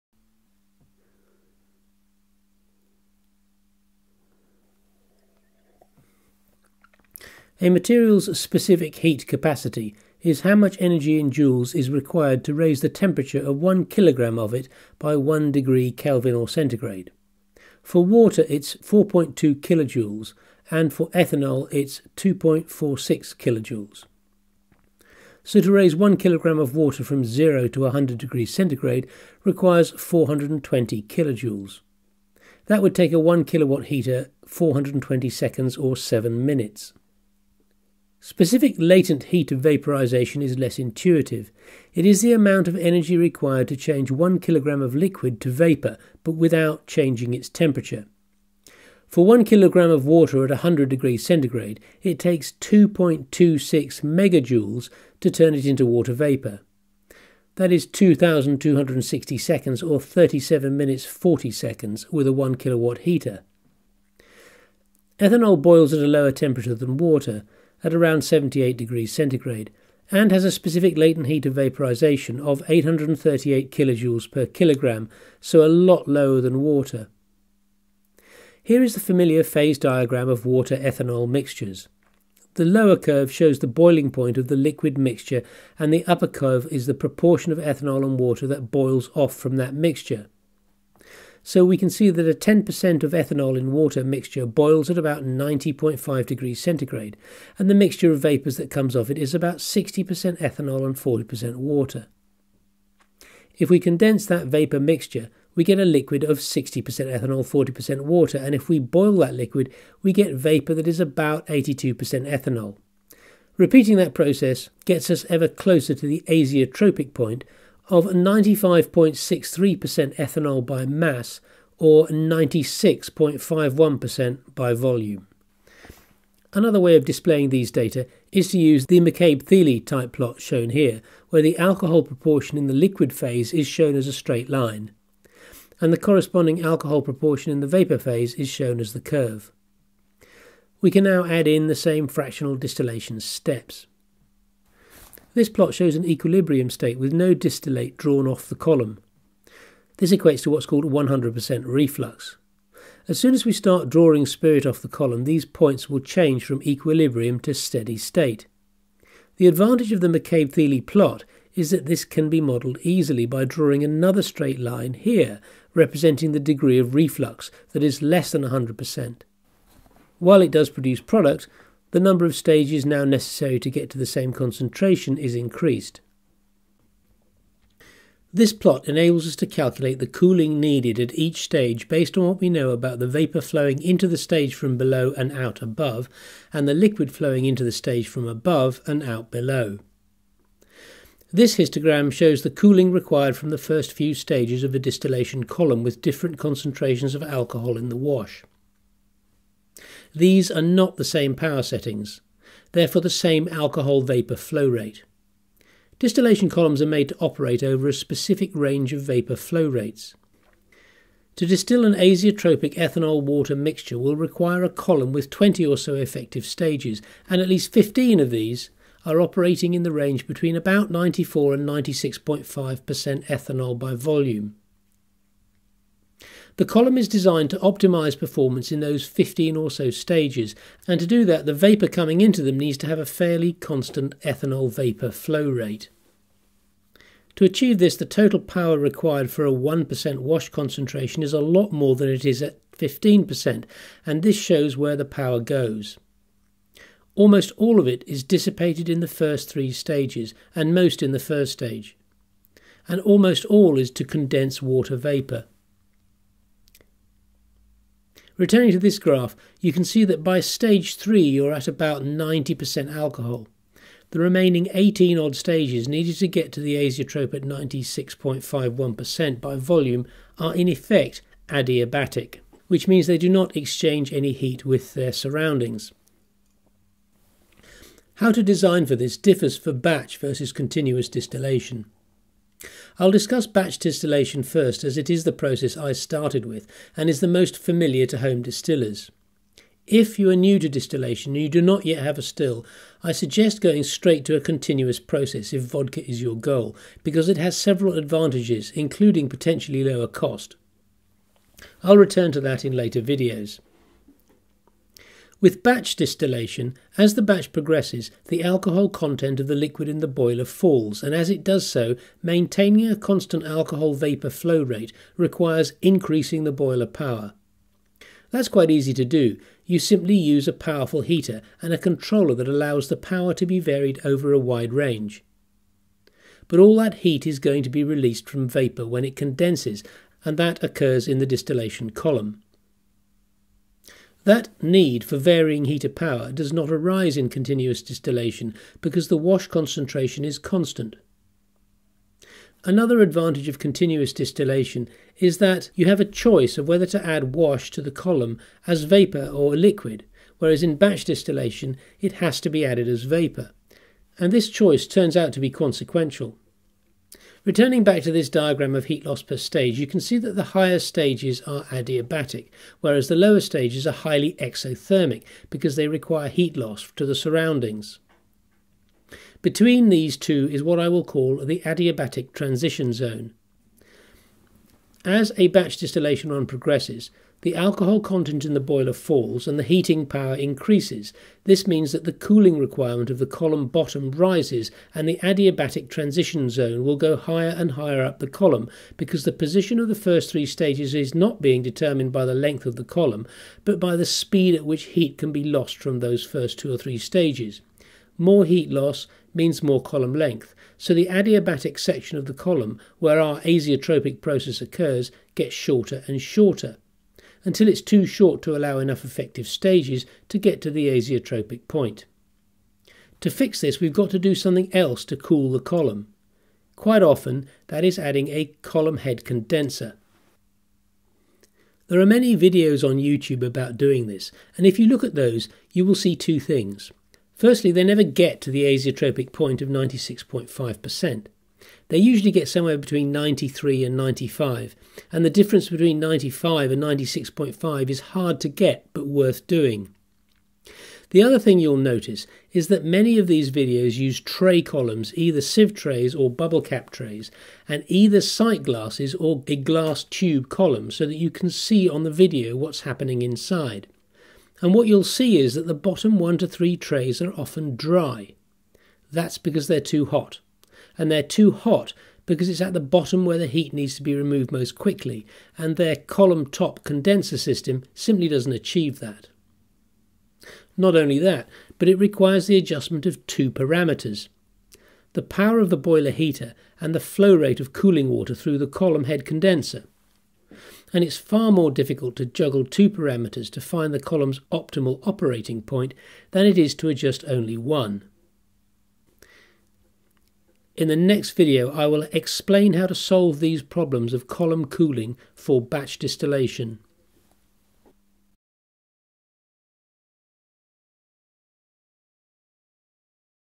Latent heat for home distillers
Essentials for high proof distillation - to 96.5% moonshine vodka